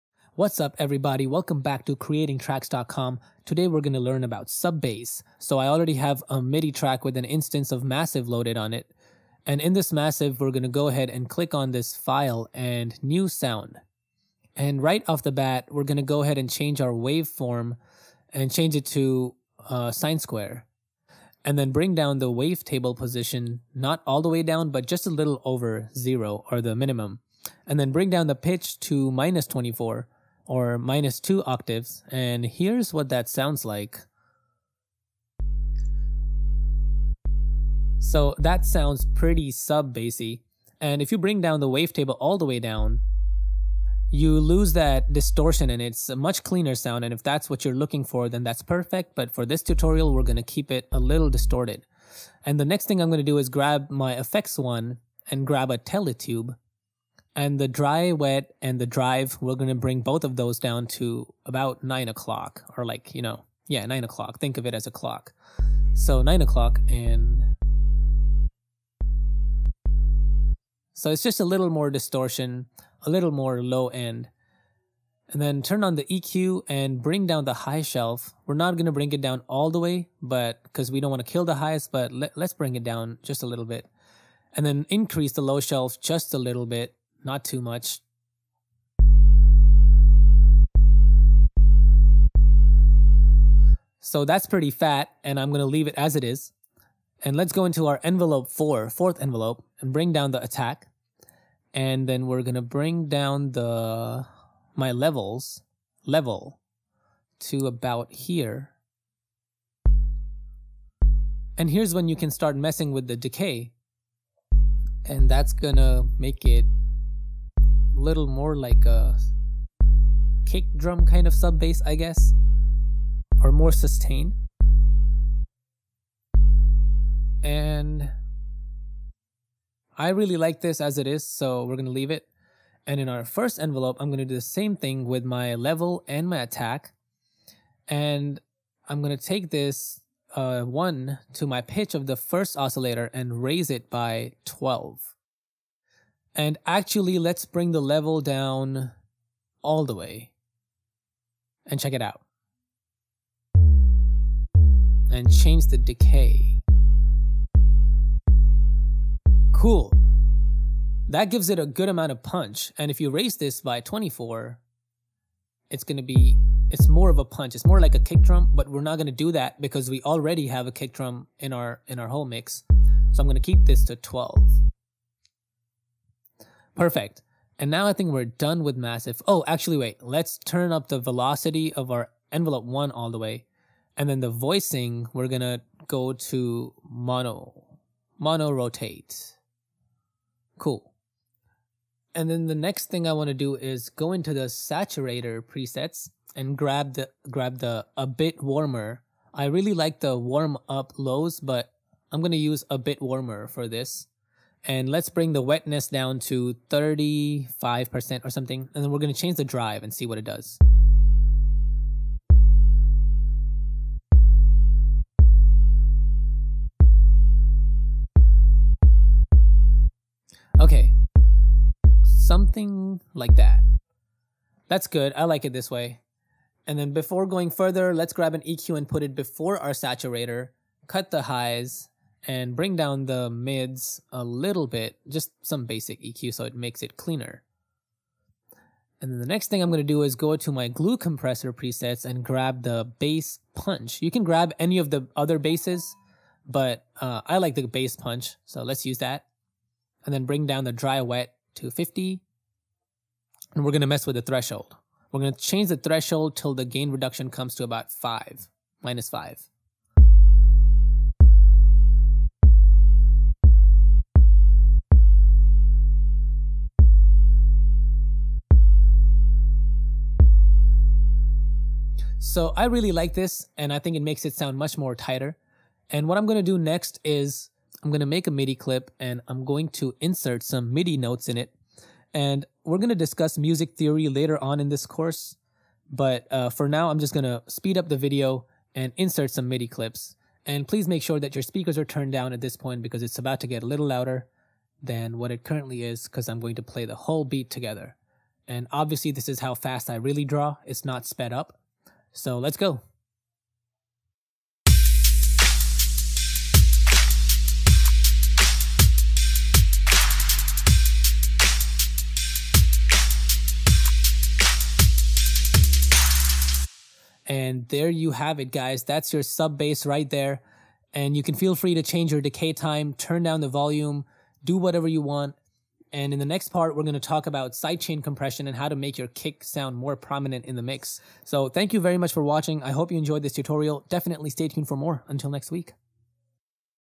Dubstep Sub Bass using NI Massive Tutorial - episode 6
In this 6th episode we look at creating a Dubstep sub bass sound.

Using NI Massive we create a sound from scratch then adjust to fit in with the track so far.